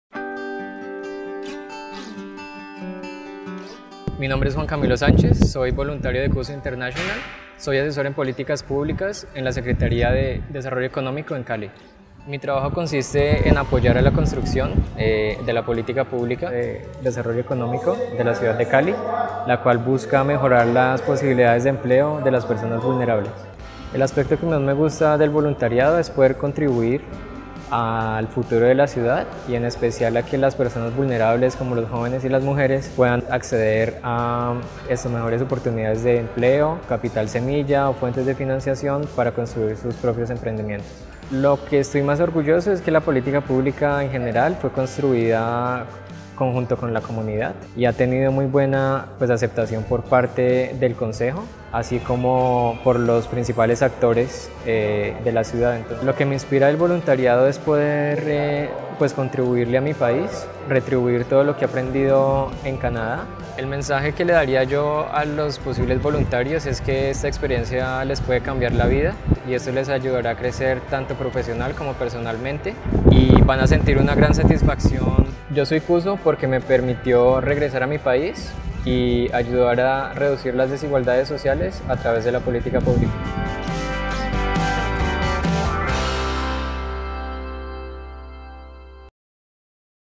IamCuso | Juan Camilo
"IamCuso because I am able to contribute to reducing social inequalities through Public Policy in Colombia, my country of origin." -Juan Camilo, Public Policy Advisor, Cali, Colombia